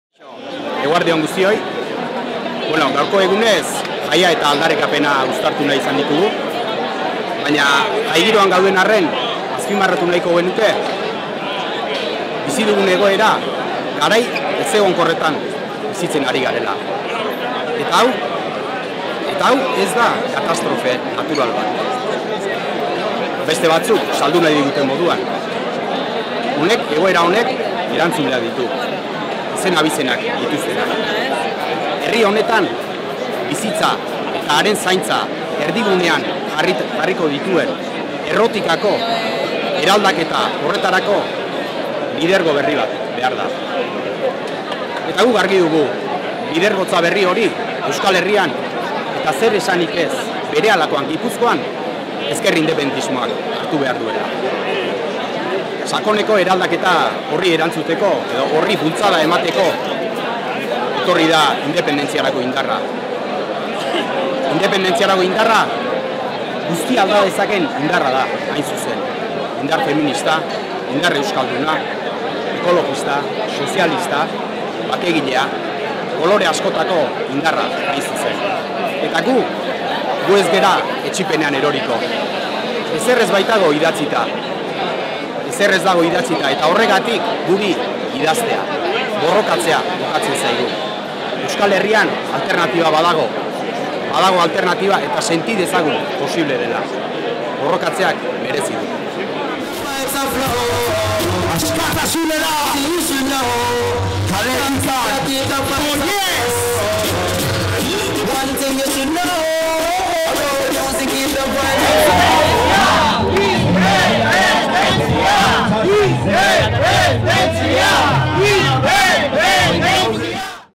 Adierazpenak | ARITZ ARZALLUS 'Independentziarako Indarra', guztia aldatzeko
Sortuk manifestazio jendetsua egin du Donostian, Kontxako estropadako bigarren igandean. Aritz Arzallus Sorturen Gipuzkoako arduradun politikoak nabarmendu du ezker independentismoak gidatu behar duela hain beharrezkoa zaigun aldaketa.